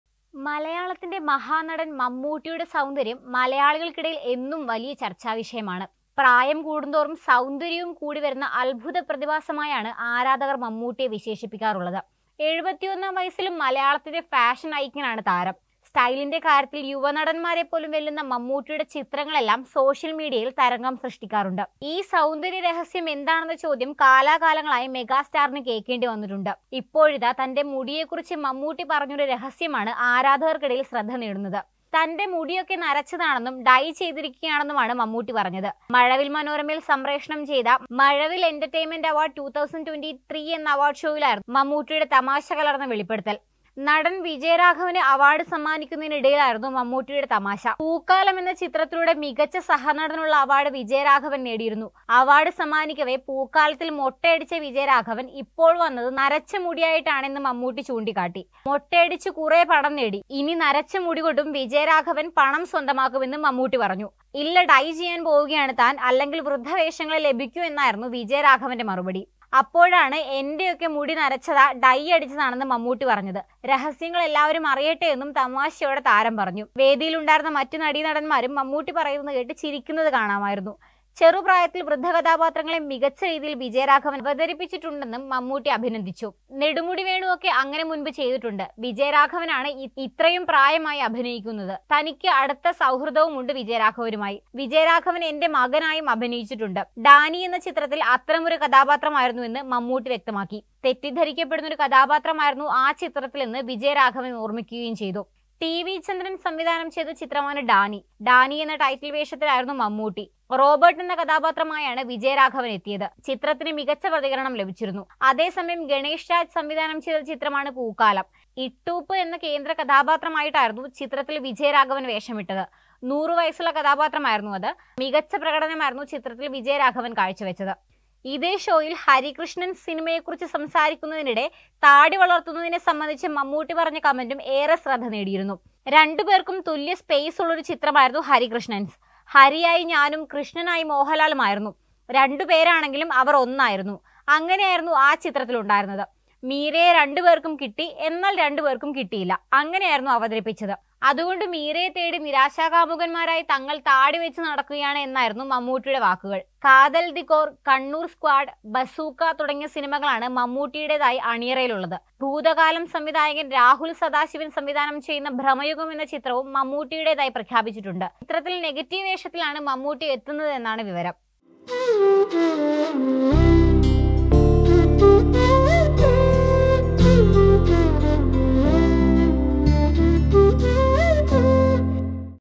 മുടിയെക്കുറിച്ചുള്ള ര​ഹസ്യം വെളിപ്പെടുത്തി മമ്മൂട്ടി | Mammootty | Indian Actor
Malayalam's great actor Mammootty's beauty is always a big debate among Malayalams. Now, a 'secret' Mammootty shared about his hair is gaining attention among fans. Mammootty said that his hair is gray and dyed. | മലയാളത്തിന്റെ മഹാനടൻ മമ്മൂട്ടിയുടെ സൗന്ദര്യം മലയാളികൾക്കിടയിൽ എന്നും വലിയ ചർച്ചയാണ്. ഇപ്പോഴിതാ തന്റെ മുടിയെക്കുറിച്ച് മമ്മൂട്ടി പറഞ്ഞ ഒരു 'രഹസ്യമാണ്' ആരാധകർക്കിടയിൽ ശ്രദ്ധനേടുന്നത്. തന്റെ മുടിയൊക്കെ നരച്ചതാണെന്നും ഡൈ ചെയ്തിരിക്കുകയാണെന്നുമാണ് മമ്മൂട്ടി പറഞ്ഞത്.

"നാടിനൊപ്പം നേരിനൊപ്പം"